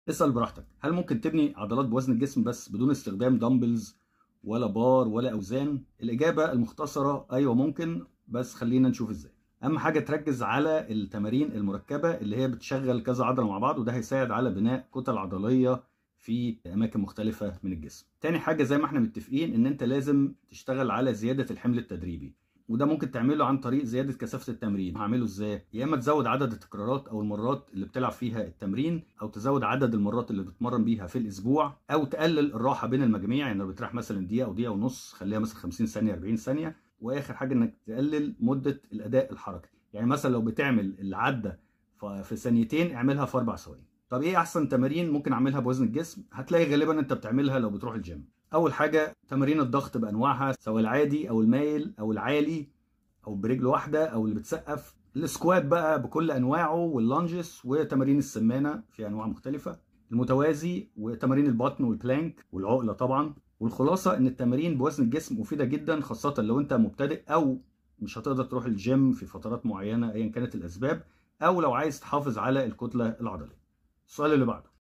بناء العضلات بوزن الجسم
إن أداء التمارين بوزن الجسم مثل: الضغط والعقلة وتمارين الارجل هي الطريقة التي يجب عليكِ اتباعها من أجل بناء العضلات بدون أوزان، لكن عليك أيضاً أن تكون استراتيجية في كيفية برمجة التدريبات لبناء العضلات بطريقة صحيحة.

← يمكنك تحميل كتيب كمال الاجسام مجانا ستجد فيه كل ما تريد معرفته عن كمال الاجسام و التغذية و التدريب..

√ Mohamed Shaalan
Certified Personal Trainer & Fitness Nutrition Specialist